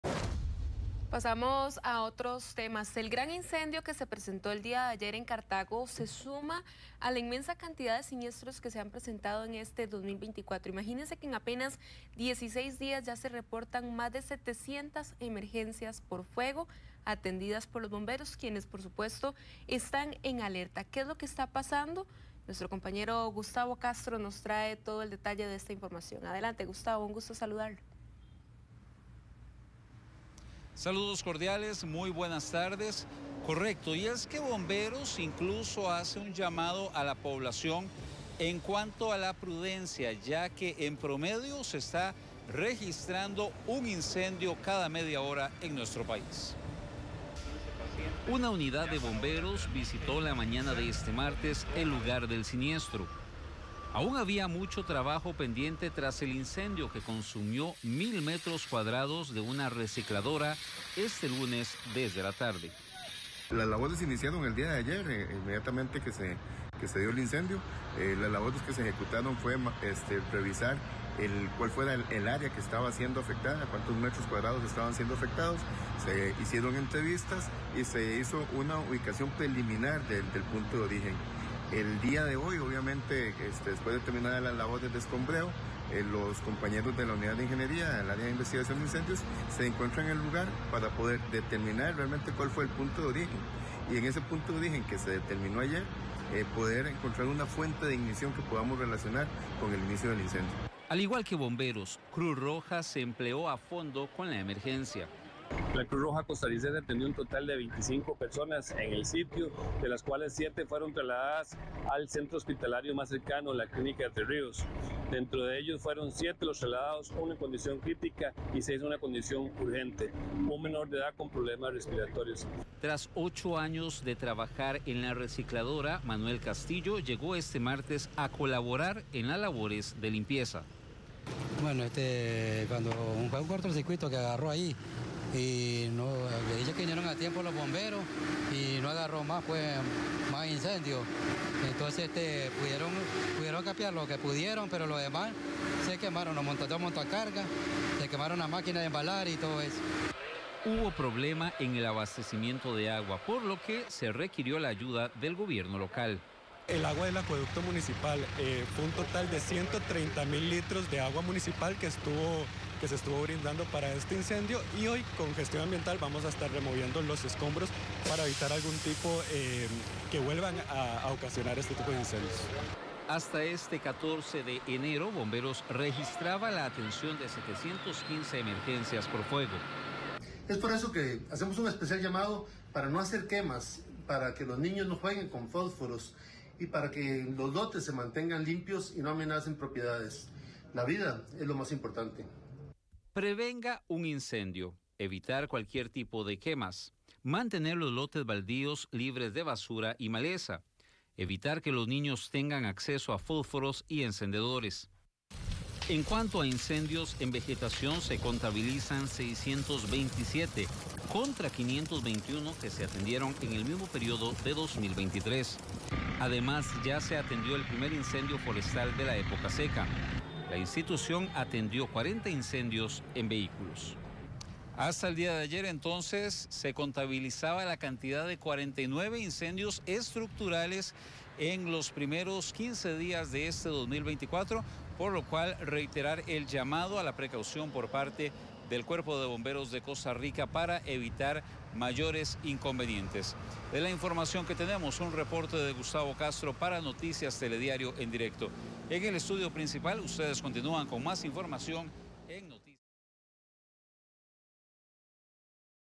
Bomberos atendieron 627 incendios en primeros 15 días del 2024
Cuerpos de emergencia registran un siniestro cada media hora.